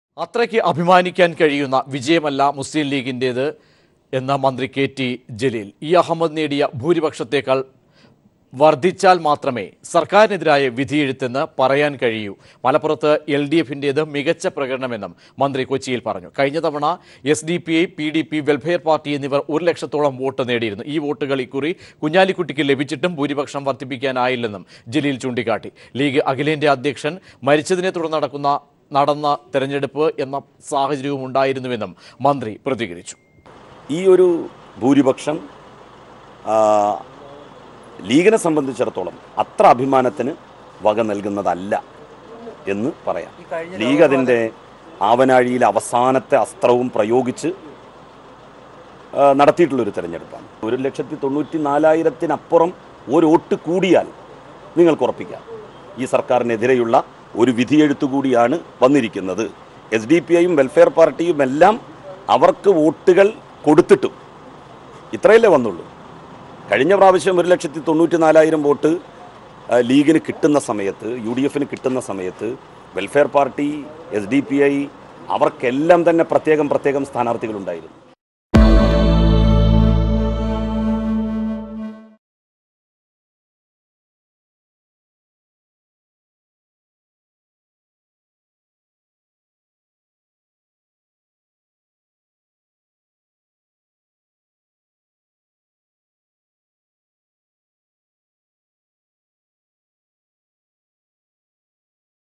Malappuram Victory Not Something That UDF Can Be Proud Of: Minister K T Jaleel
Minister K T Jaleel said that the UDF's victory at Malappuram is not something that they can be proud of as the majority went down by a large margin.